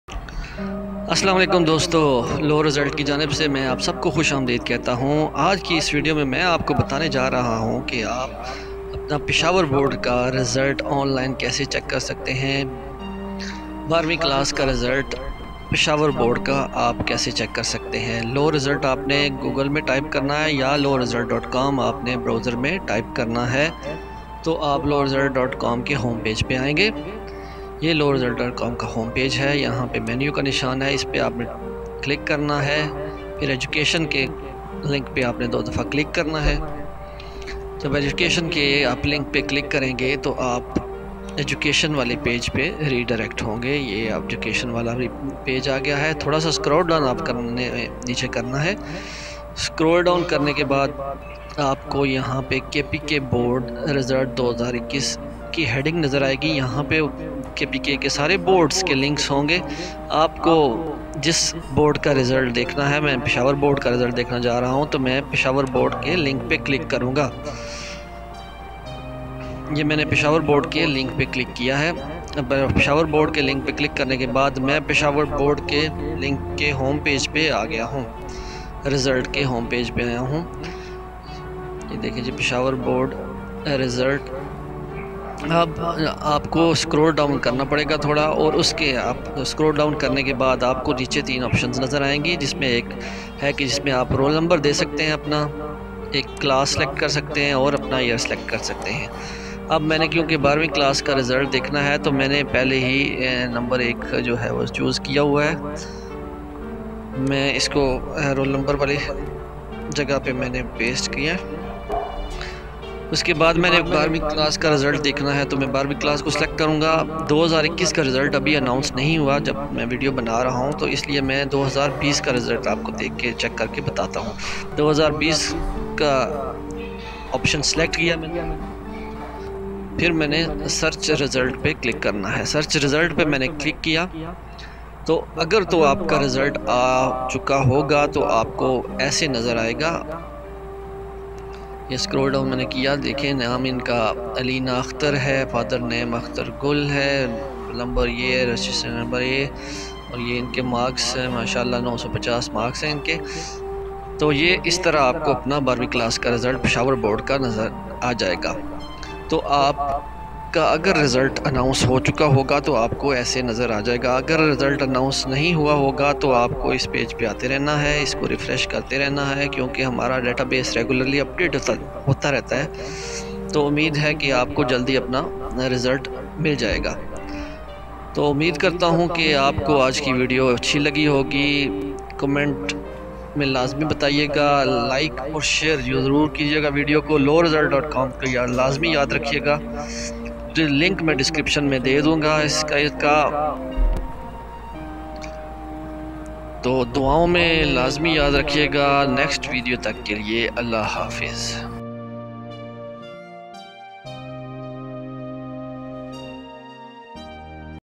Peshawar Board 12th Class Result 2022
result result2022 Peshawar Board 12th Class Result 2022 ka result check karne ka tarika. 2nd year, HSSC part 2, Inter part 2 result online of BISEP BISE Peshawar.